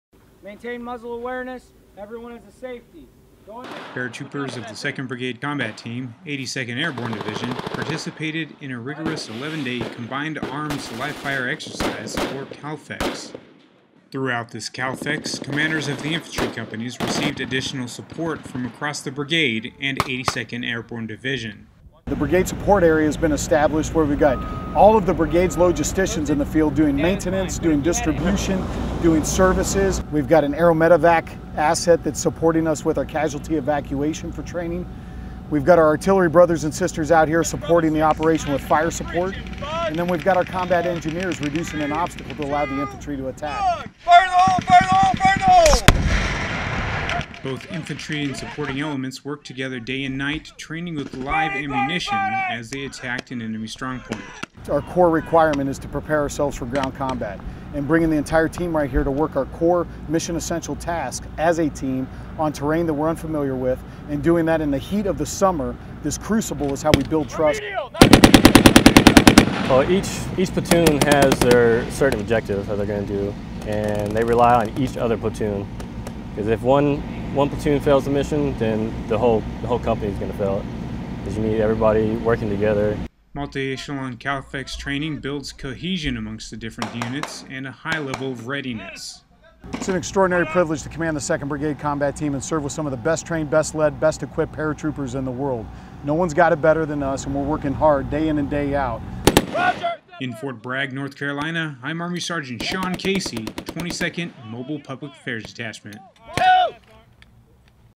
CALFEX
Paratroopers assigned to the 2nd Brigade Combat Team, 82nd Airborne Division, conducted a 10-day Combined Arms Live Fire Exercise (CALFEX) on Fort Bragg, N.C. The BCT conducted the CALFEX in preparation for battalion-level live fire exercises later this year.